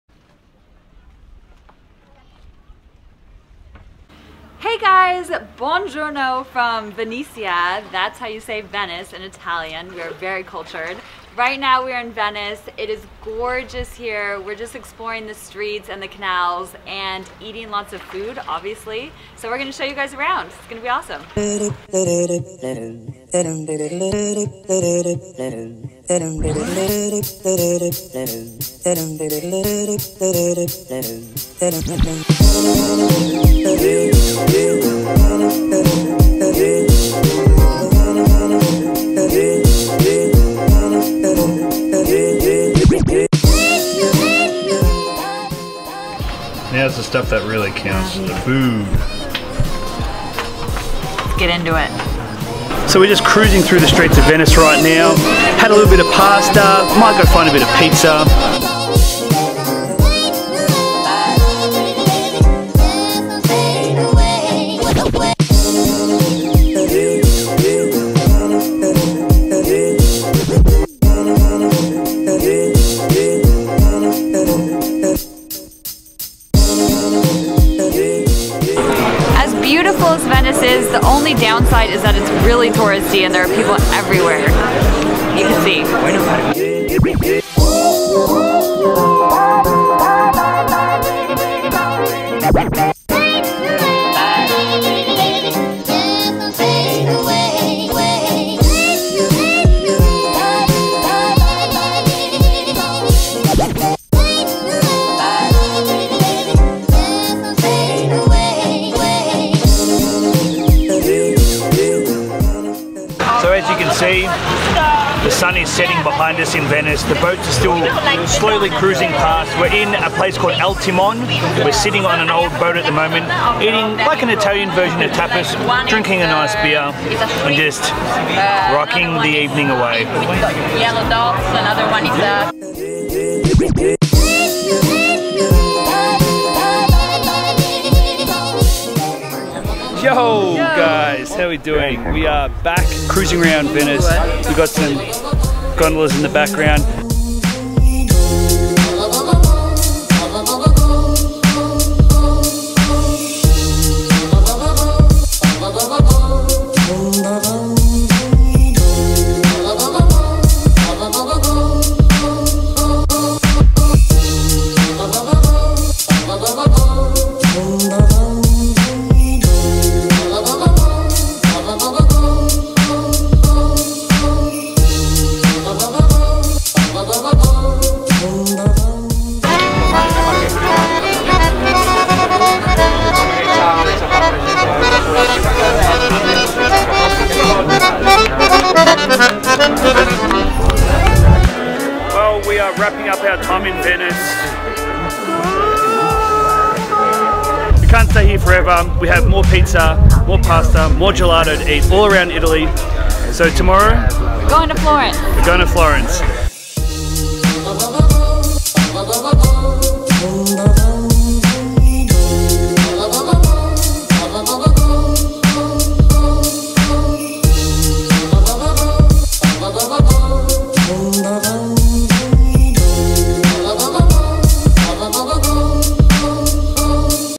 OUR FIRST VLOG... FROM THE CANALS OF VENICE, ITALY!
Kicking off the first official Vlog from Jules and Christine at Don't Forget To Move! And what better place to do it than in Italy!

Join us as we explore the canals of Venice in Italy, taking in all the art, culture and food that we can!

Never miss an adventure with Don't Forget To Move